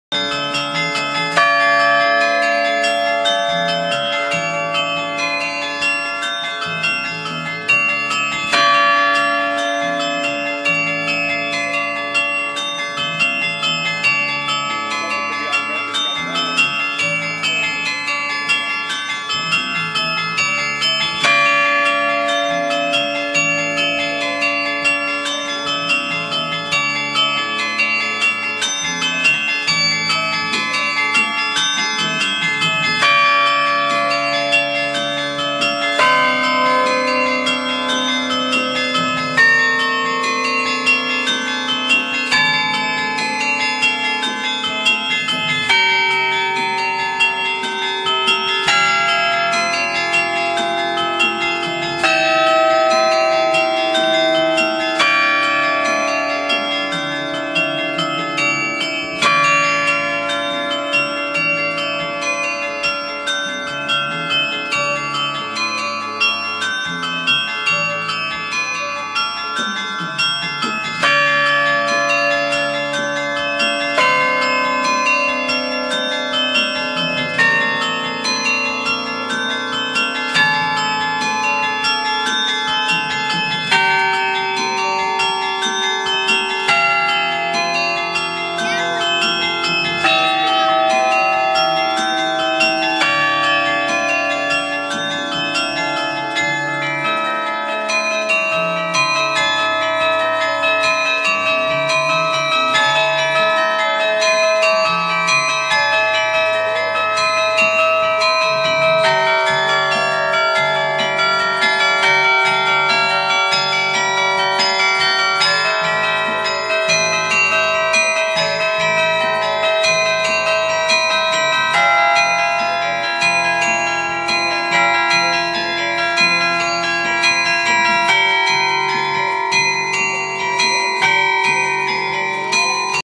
Cast in Bronze at the Colorado Renaissance Fair
Frank DellaPenna playing the Carillon at the 2012 Colorado Renaissance Fair near Larkspur Colorado